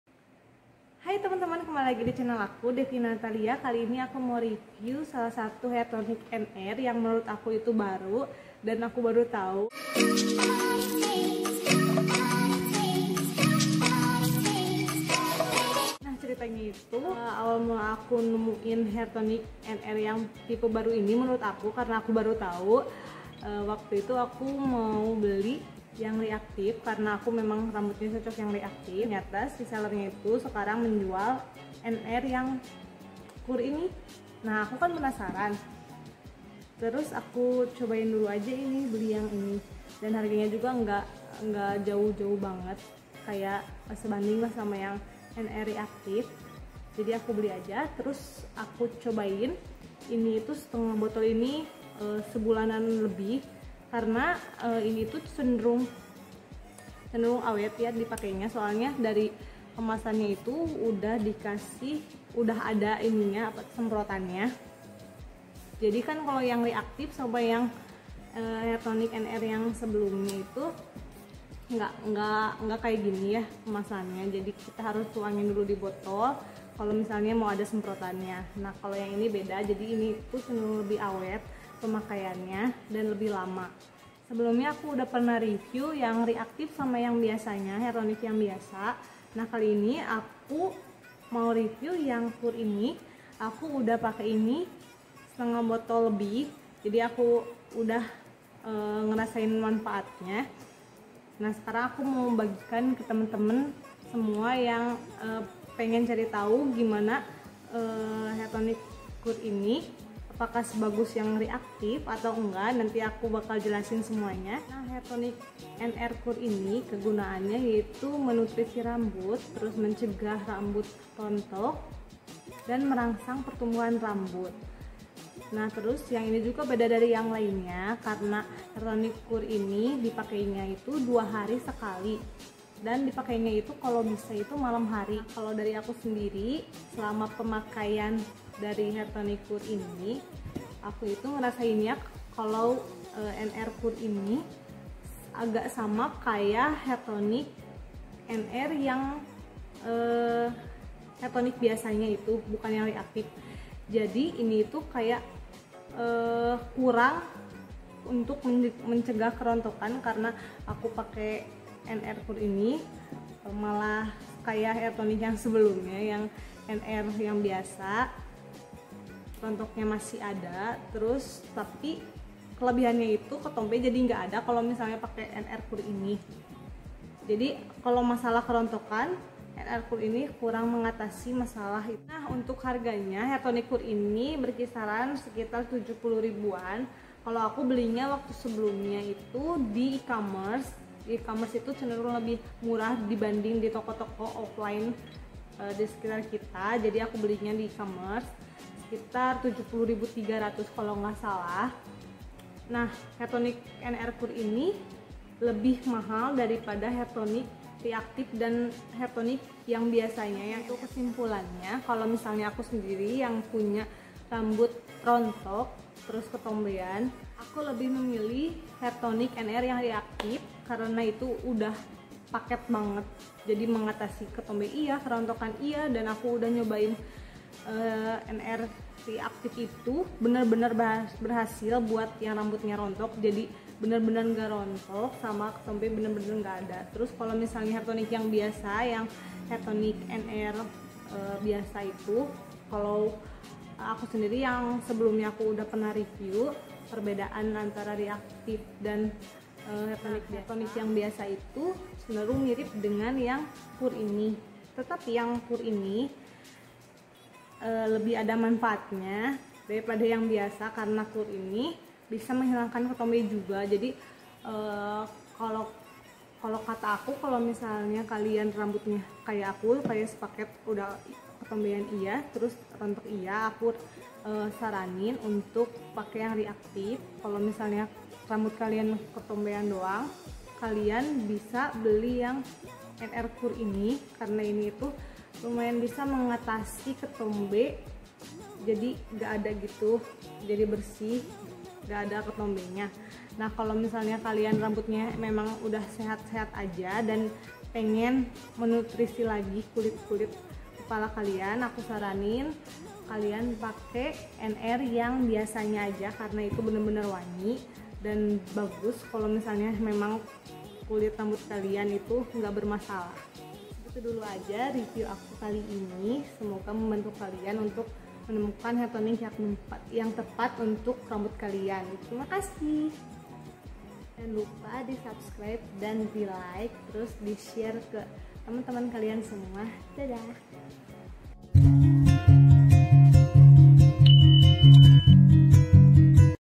REVIEW HAIR TONIC NR KUR + PERBEDAAN DENGAN HAIR TONIC NR LAINNYA - Dvna Natalia
BELI DISINI :
•SHOPEE

Haii Temenn-Temenn Youtube 💕💕
Makaasii udah Nontonn 💕